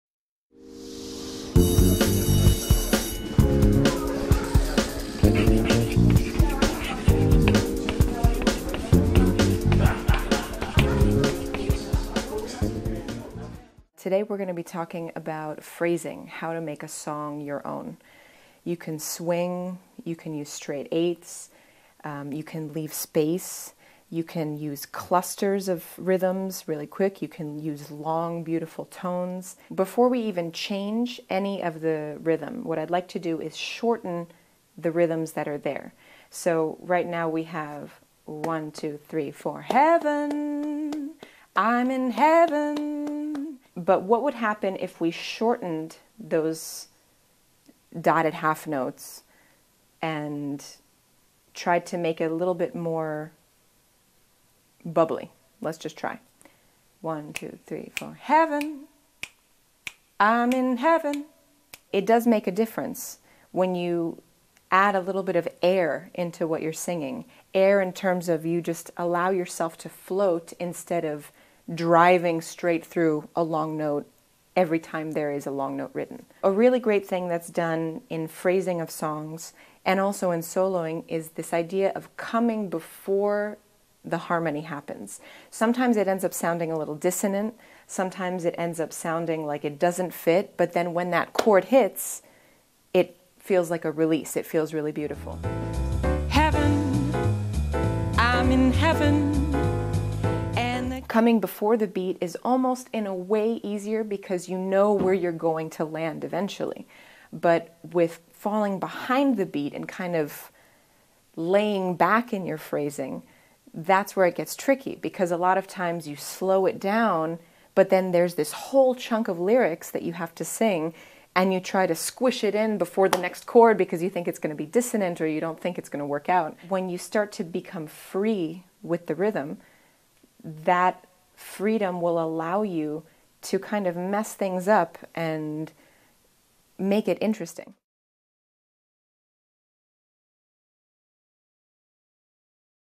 Thana Alexa - Jazz Vocal Phrasing
Description:In this jazz vocal phrasing lesson, singer Thana Alexa shows you how to make a song and/or standard your own. Thana discusses and demonstrates different phrasing concepts which will allow you to freely sing across the bar line, add tension and release, purvey specific moods/feeling and more. If you want to improve your phrasing when singing jazz, this voice masterclass is for you.

Topics Covered: Vocals, Jazz, Phrasing, Singing Standards, Articulation, Note Lengths, Anticipating the Beat, Delayed Resolutions, Dynamics, Mood, Making a Song Your Own, etc.